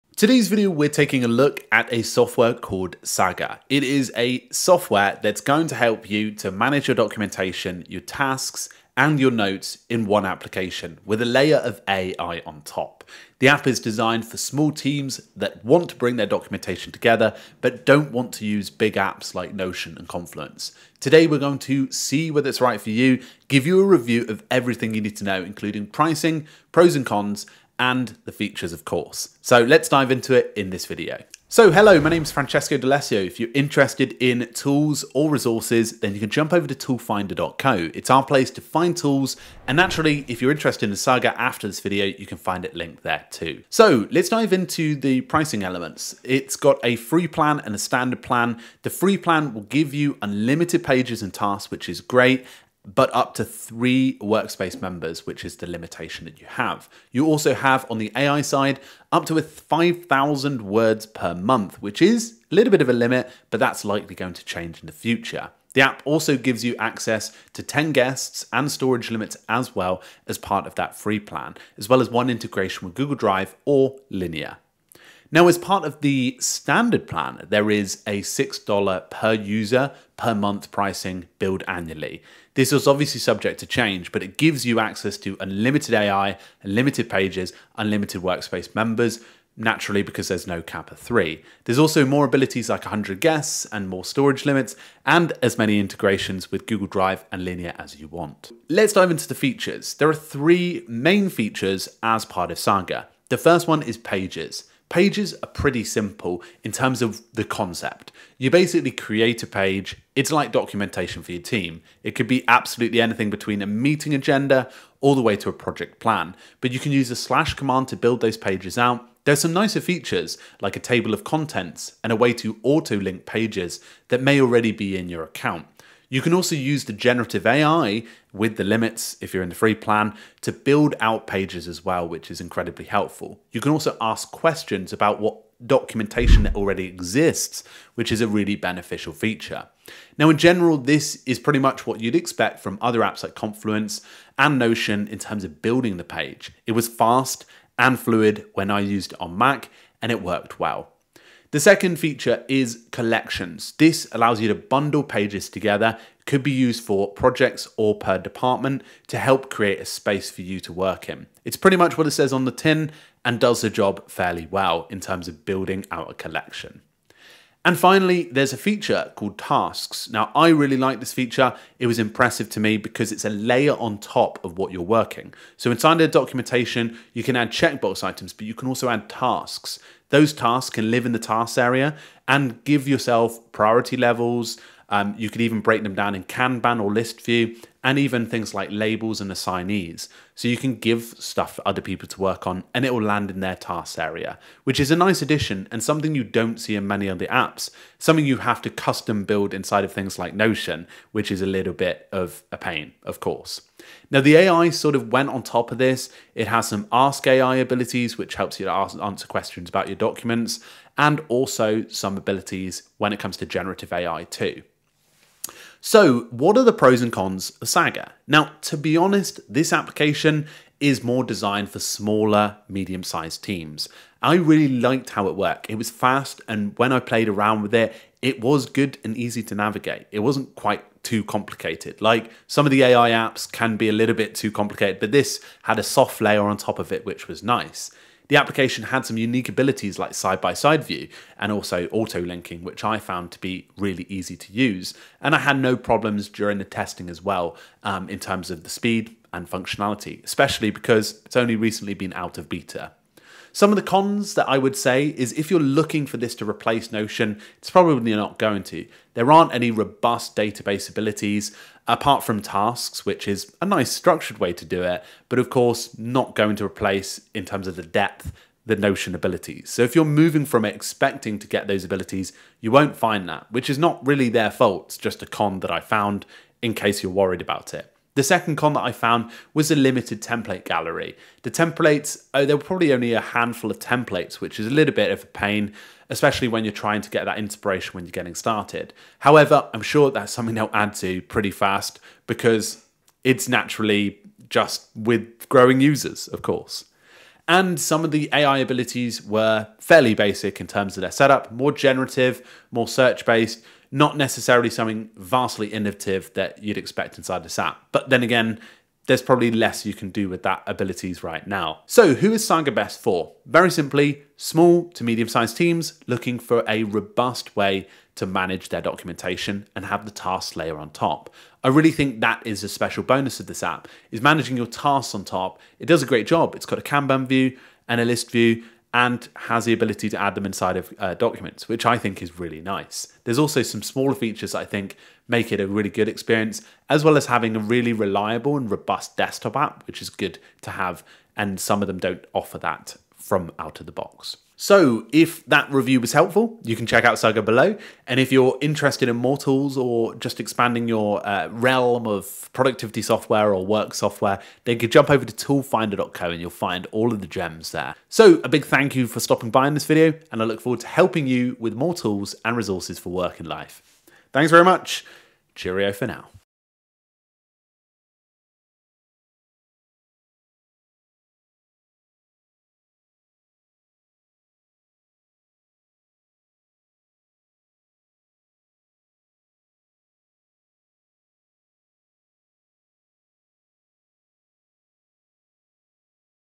Saga AI: Is this Your Team's AI Note-Taker?
Stop doing busywork! Try Bento Focus Teams seeking out team wikis, Saga would be your next base, let's review it.